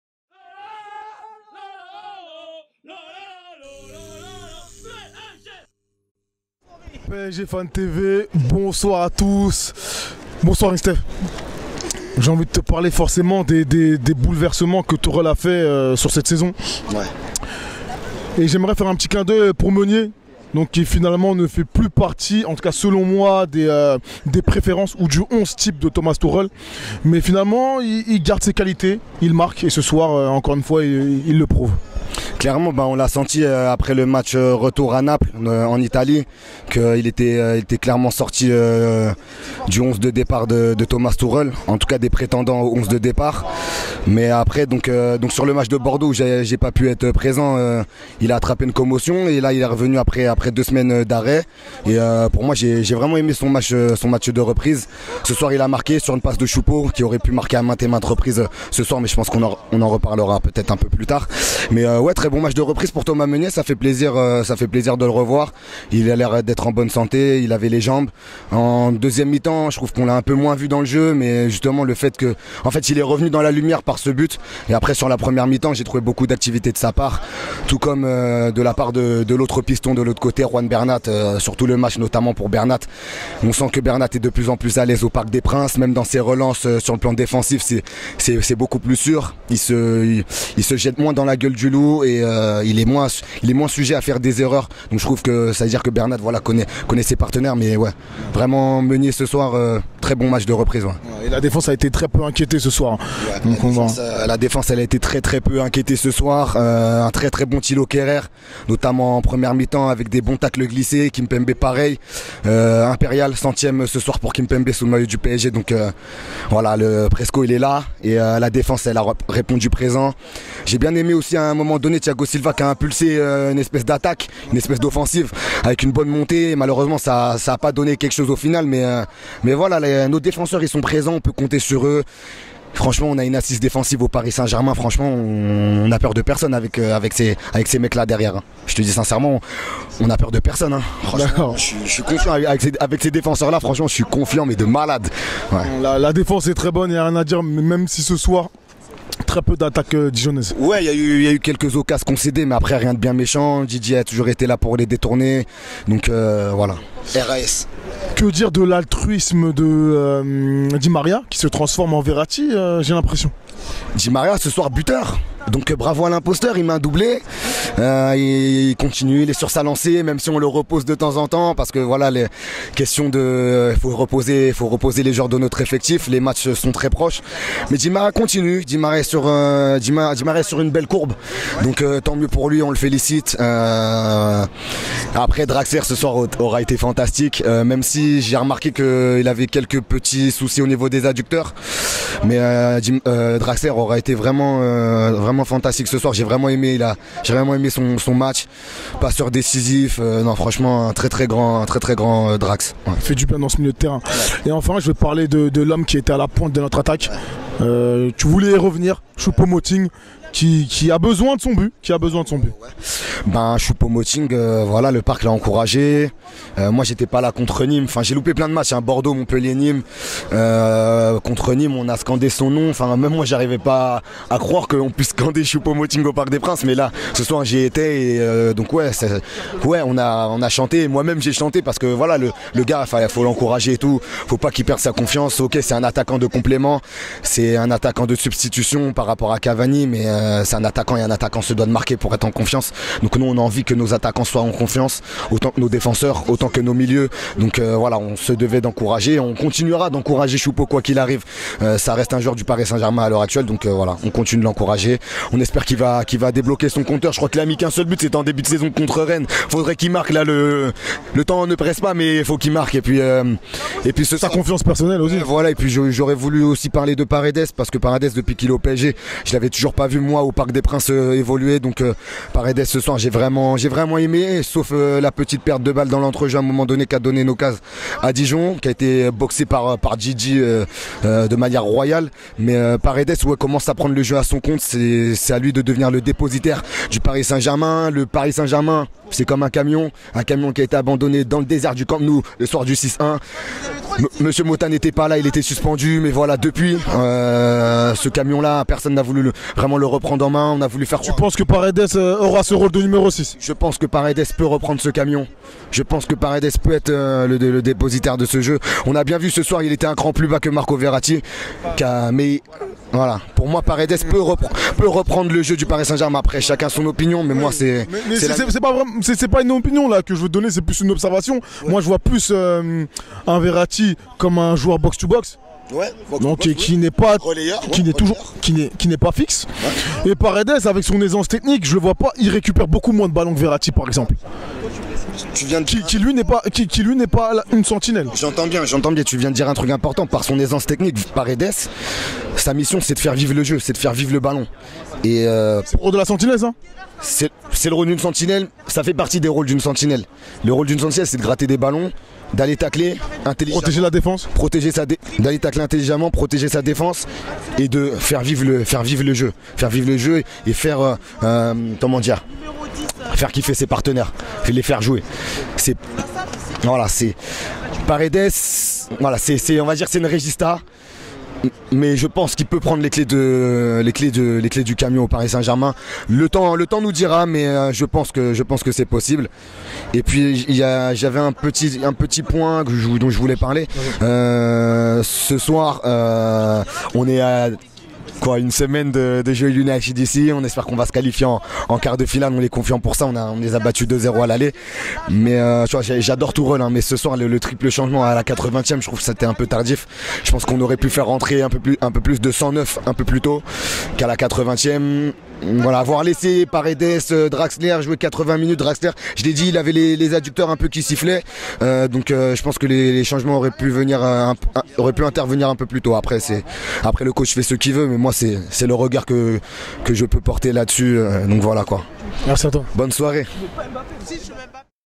PSG vs Dijon 3-0 | Paredes Peut Prendre Les Clefs Du Camion ! (Yungsteph)
Les supporters parisiens analysent la rencontre du Paris Saint-Germain face à Dijon, lors des quarts de finale de la Coupe De France. Un match contrôléde la part des bleus et rouge. Les dijonnais n'ont pas été offensifs.

C'est Parti !!